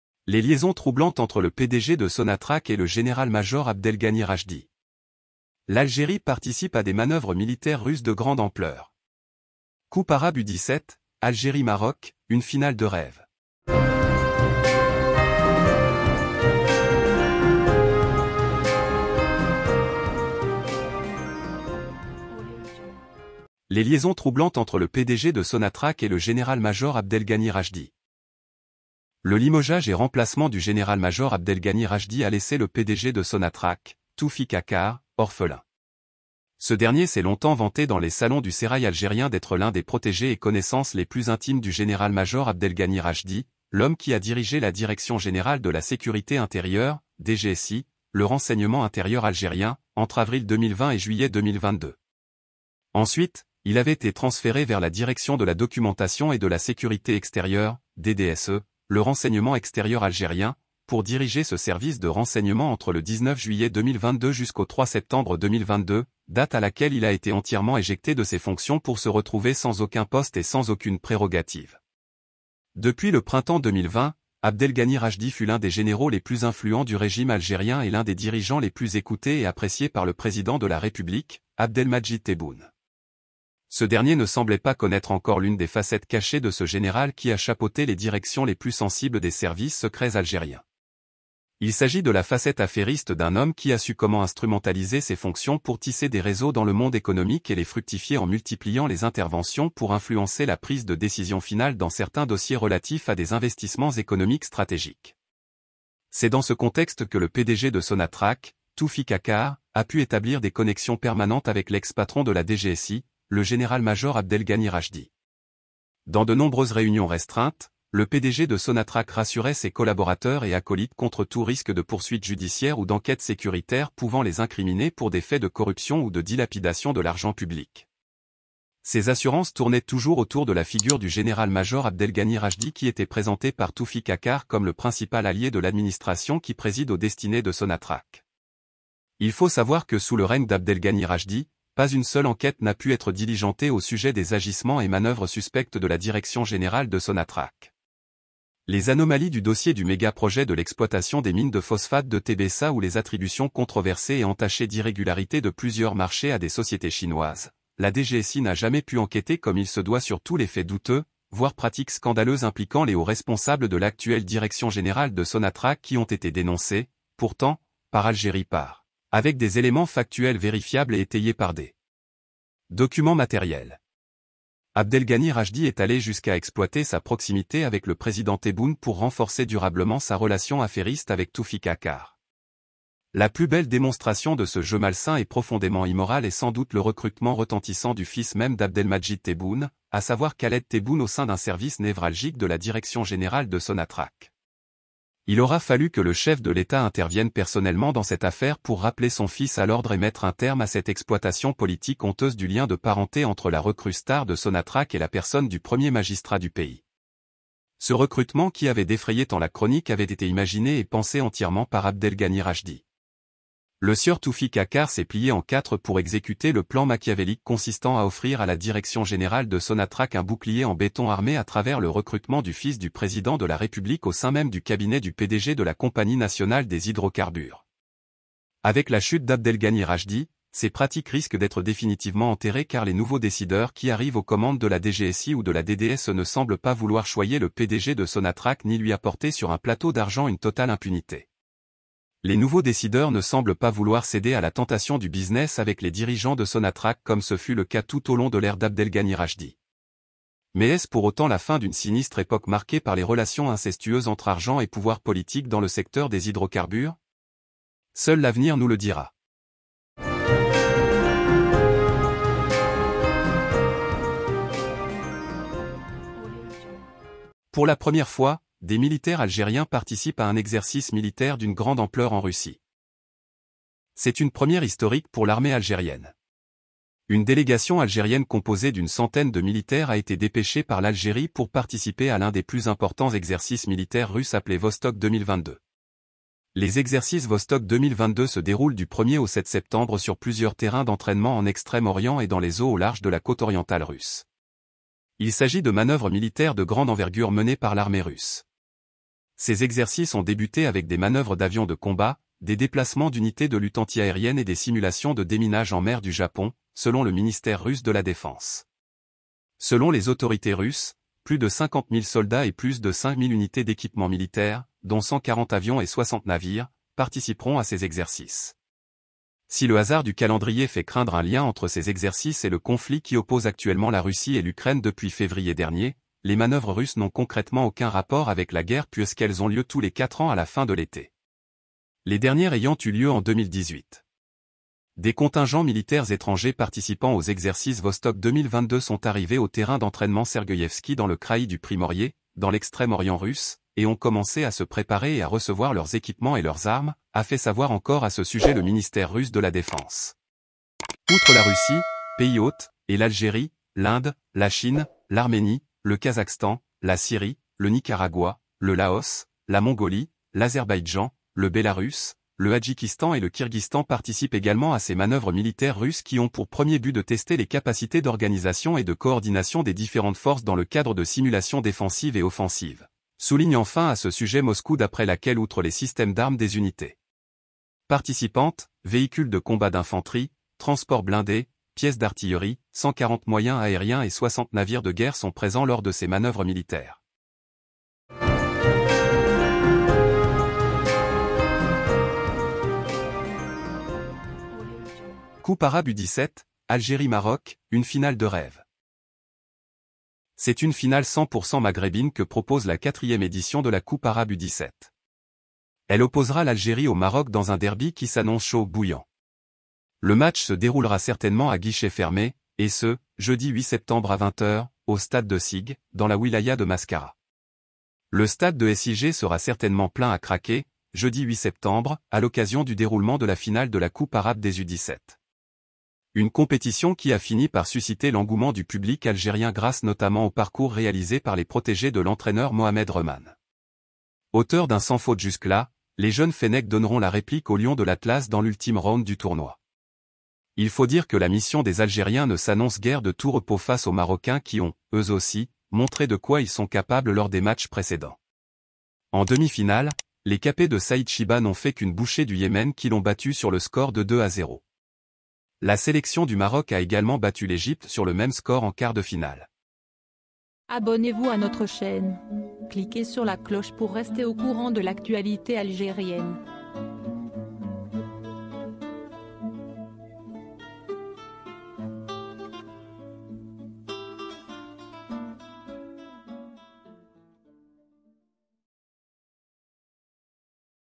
Les liaisons troublantes entre le PDG de Sonatrach et le général major Abdelghani Rachedi...
L'Algérie participe à des manœuvres militaires russes de grande ampleur !
Coupe arabe U17 : Algérie-Maroc, une finale de rêve !